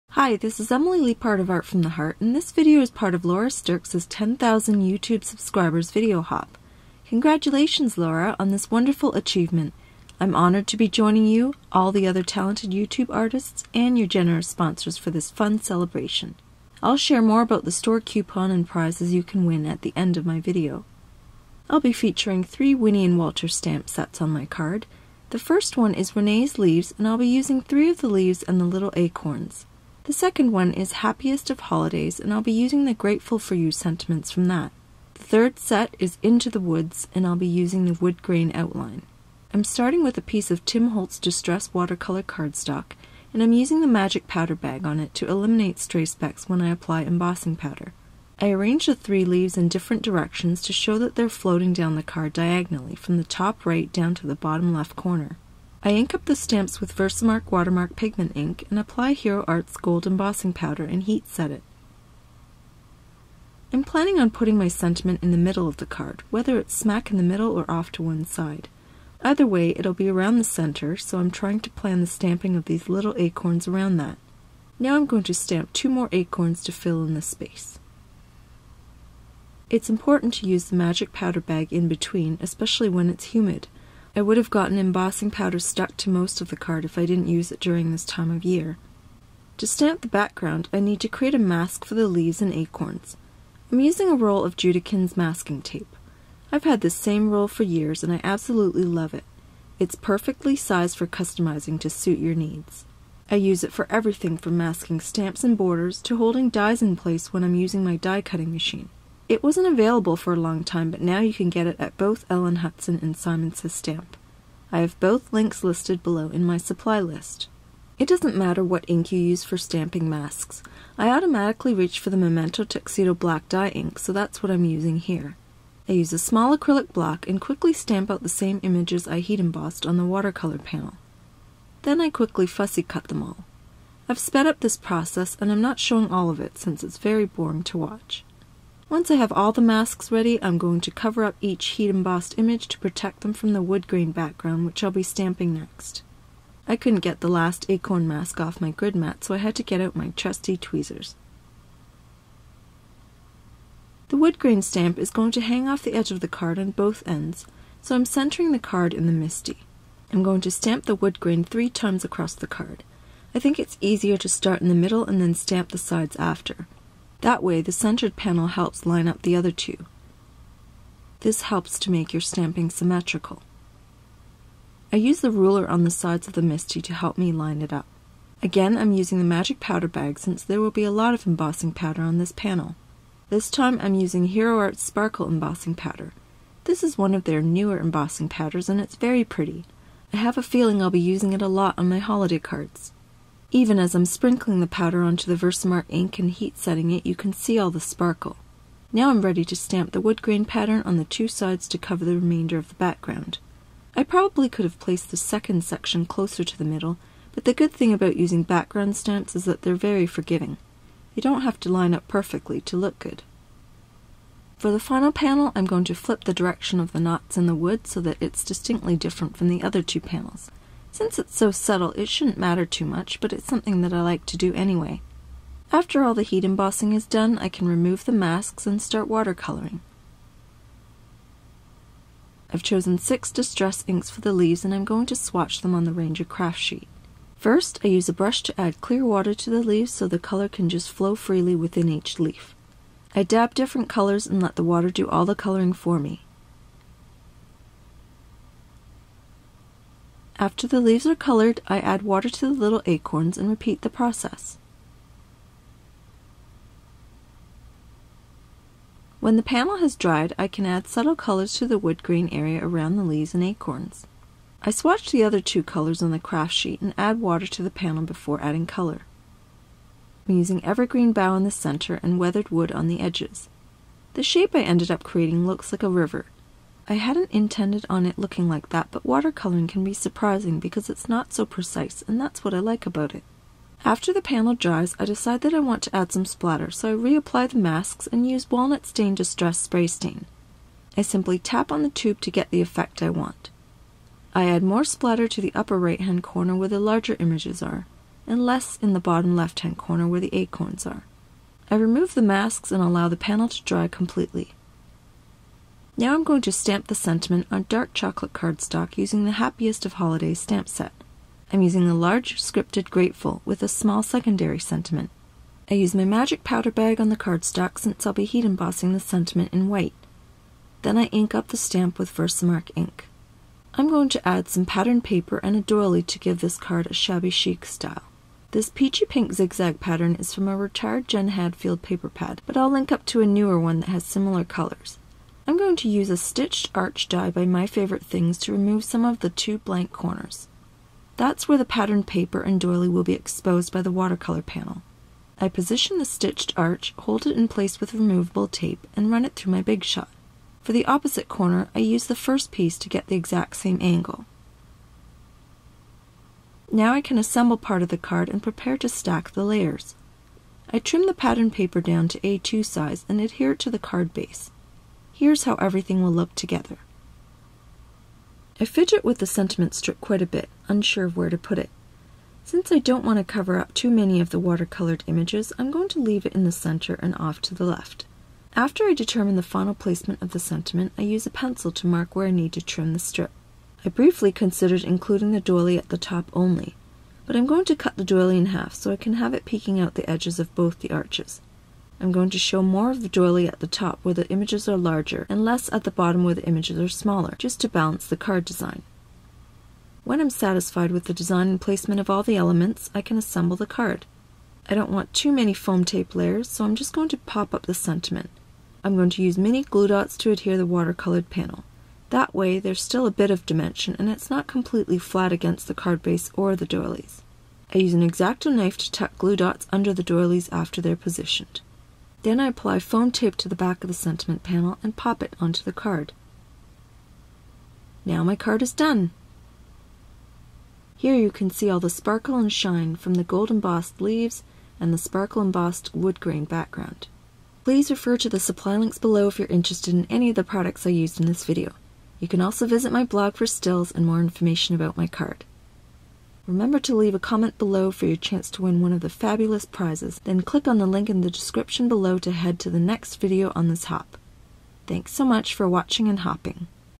Laura Sterckx's 10,000 YouTube Subscribers Video Hop + COUPON + PRIZES!!
This video is part of Laura Sterckx's 10,000 YouTube Subscribers Video Hop and includes generous prizes and a store coupon. For a chance to win, please leave a comment at every video on the hop!

• Winnie and Walter HAPPIEST OF HOLIDAYS EVELIN T DESIGNS Clear Stamps WW049  ——
• Ranger Inkssentials Non Stick CRAFT SHEET Reusable 15 X 18 Stamp Paint Ink  ——